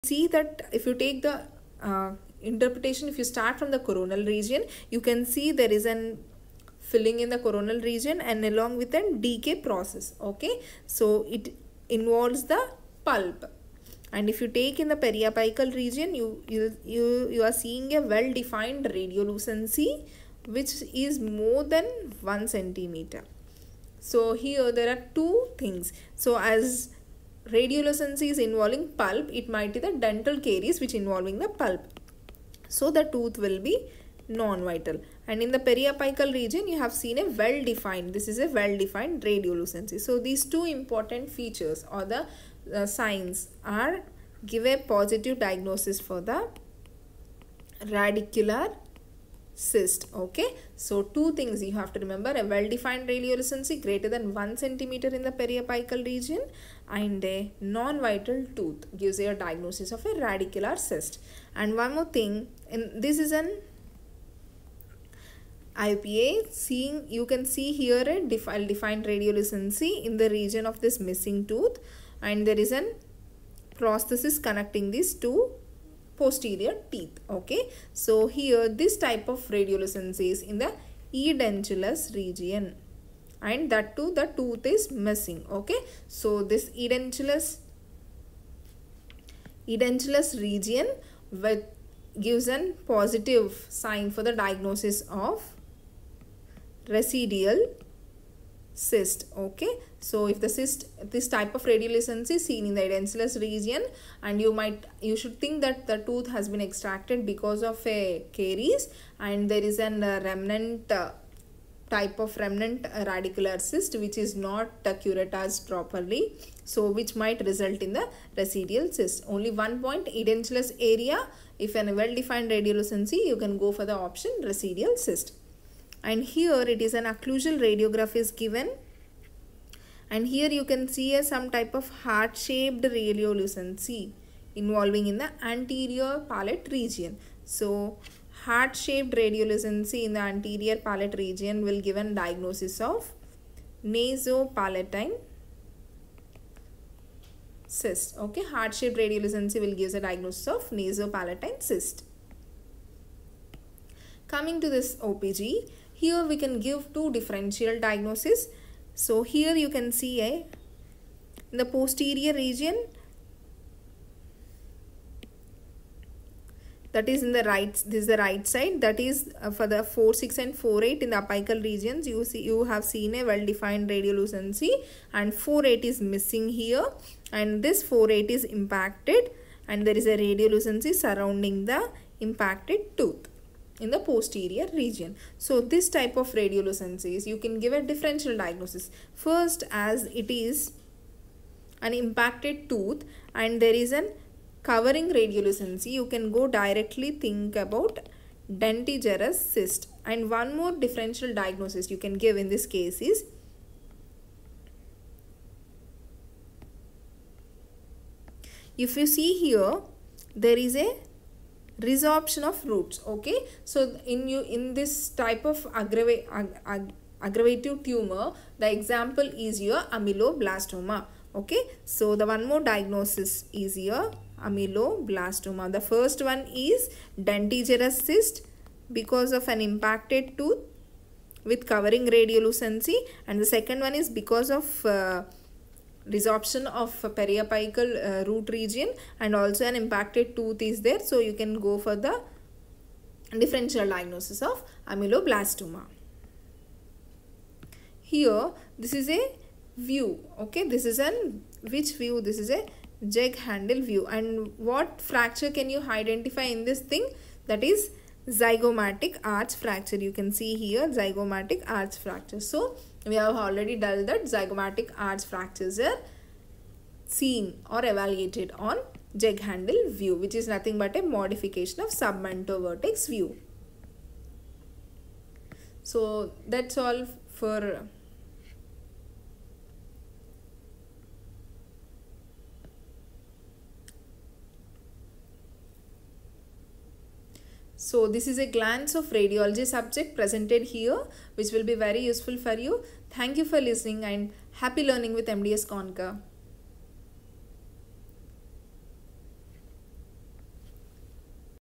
RADIOLOGY PART 2 II NEET MDS || INICET MDS || Quick Revision | WE ARE WITH YOU - TEAM MDSCONQUER
RADIOLOGY PART 2 II NEET MDS || INICET MDS || Quick Revision
By Dr.G Sahithi Dattar TEAM MDS CONQUER.

WE ARE WITH YOU ALWAYS TEAM MDSCONQUER
Happy learning with MDS CONQUER :-)